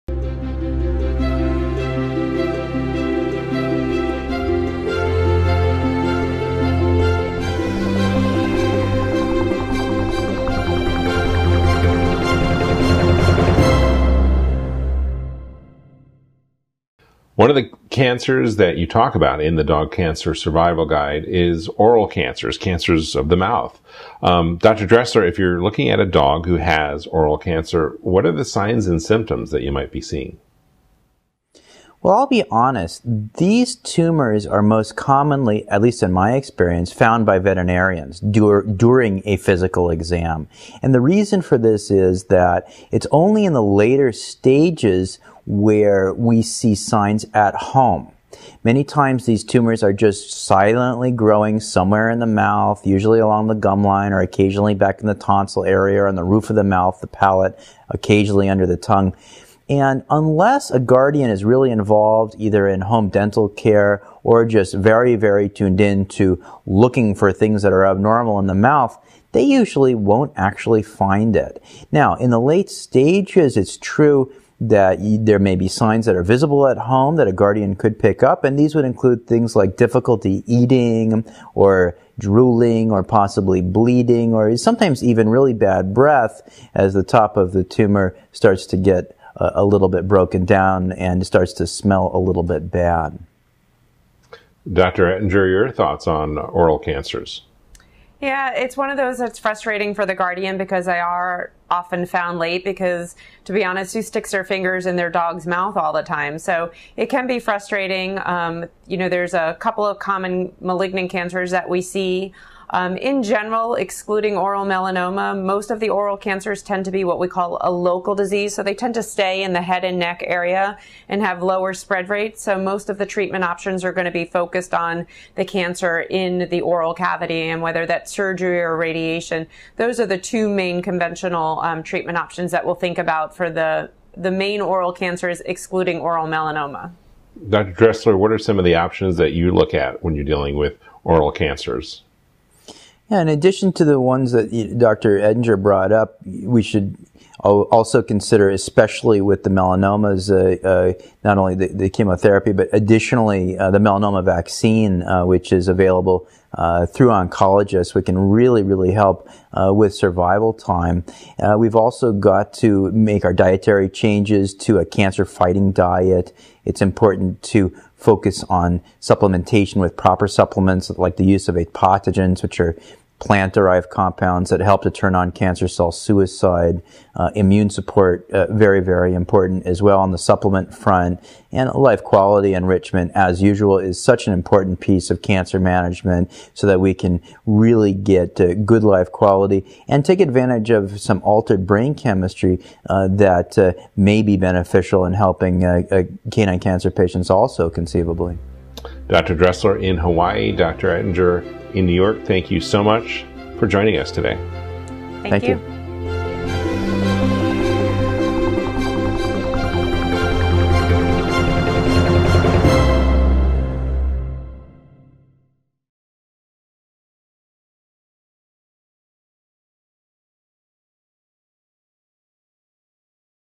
What to Know About Oral Cancers
Dr. Dressler and Dr. Ettinger talk about the detection, diagnosis, and Full Spectrum Care Approach to the treatment of oral cancers in dogs.

Topics Covered:
Signs and Symptoms of Oral Cancer
Spread Rate
Conventional Treatments: Surgery and Radiation
Melanoma vaccine
How to address oral cancer with a Full Spectrum strategy

Get to know today's guests:
Sue Ettinger, DVM, Dip. ACVIM (Oncology):